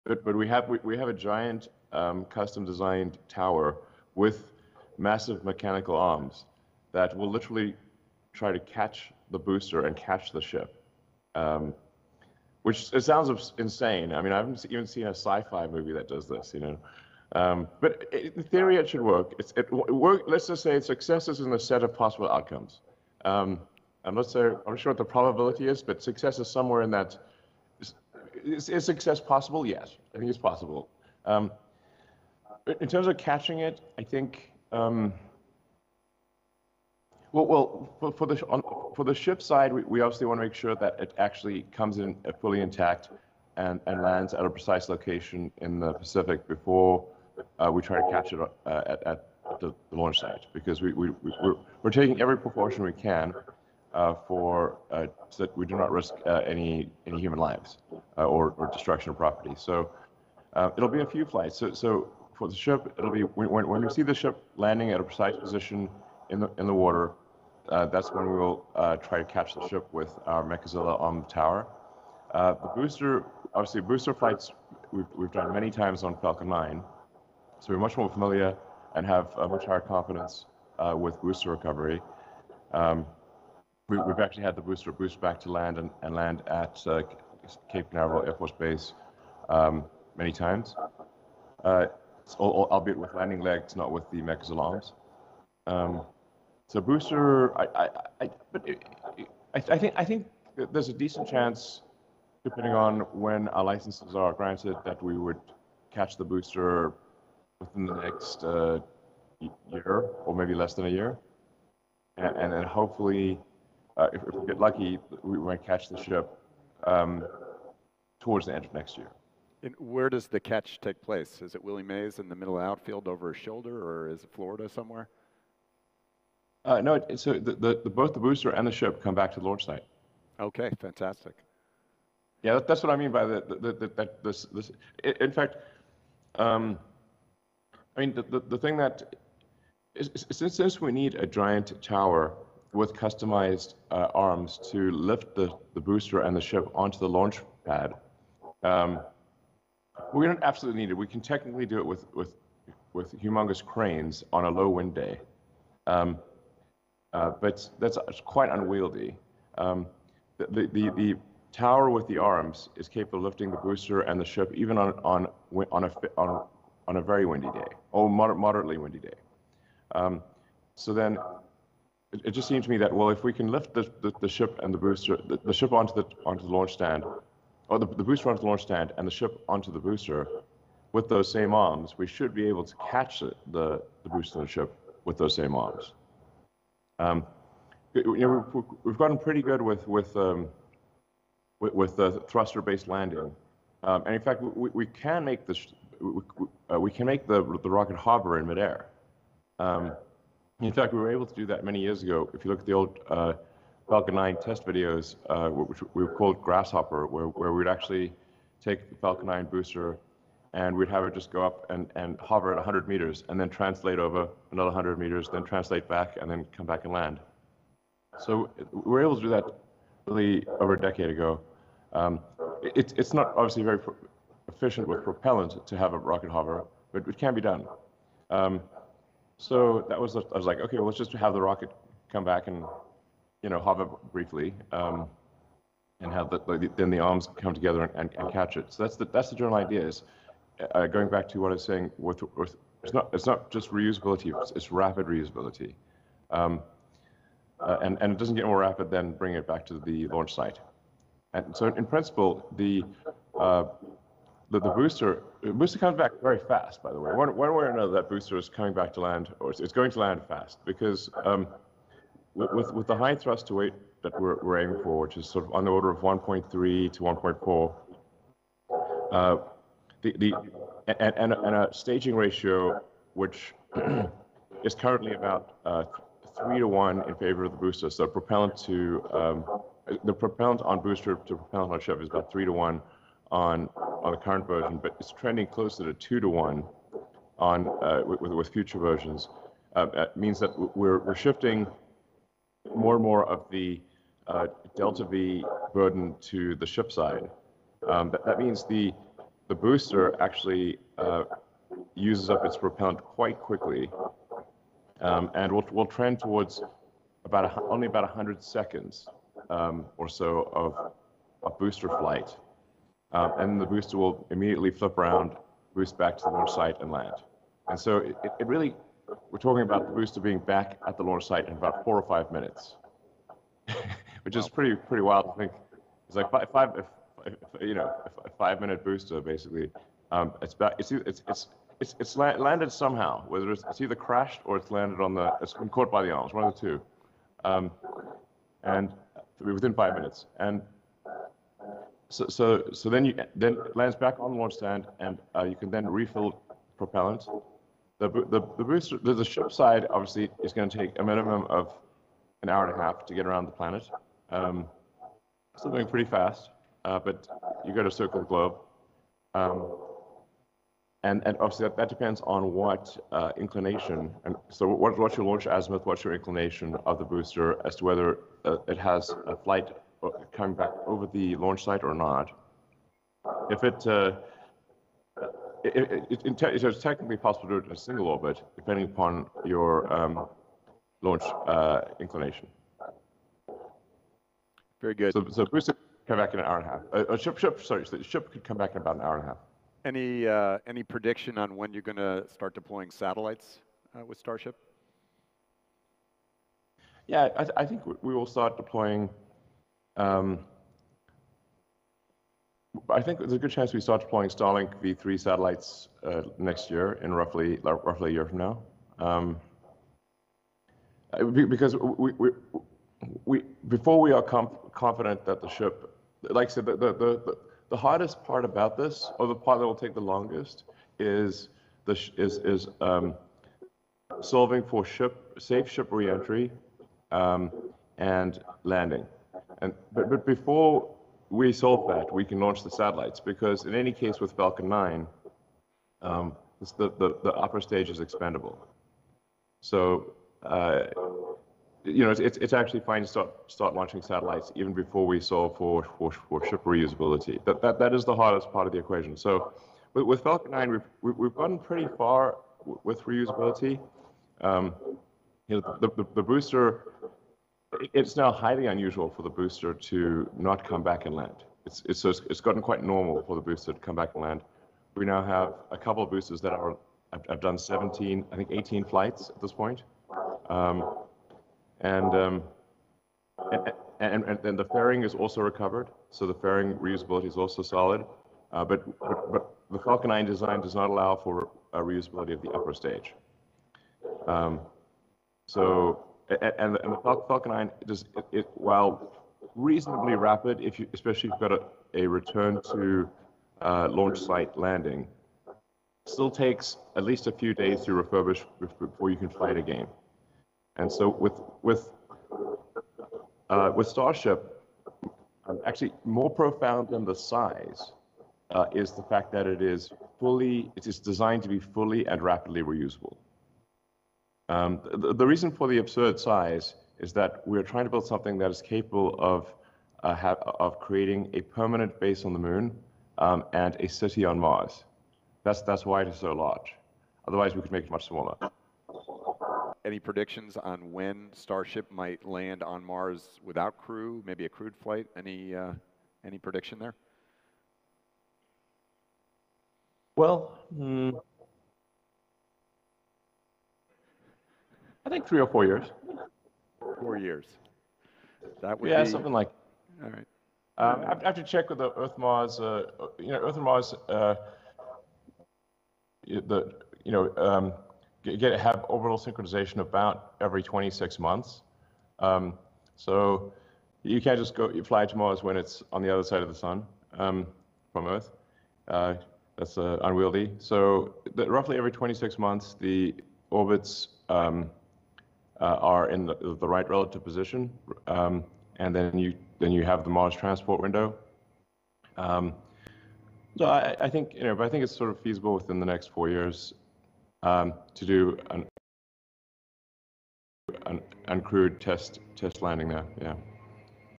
Elon Musk's SpaceX Starship update at IAC 2023 - Starlink launches, booster catches and Mars!
Elon Musk is hopeful SpaceX will launch Starlink satellites using Starship in 2024 along with attempting to catch the Super Heavy booster and spacecraft with the launch tower's mechanical arms. He also gives a "3 to 4 year" estimate for a Starship landing  on Mars.

Musk delivered the update during the 74th International Astronautical Congress (IAC) on Oct. 5, 2023.

Credit: IAF / Azercosmos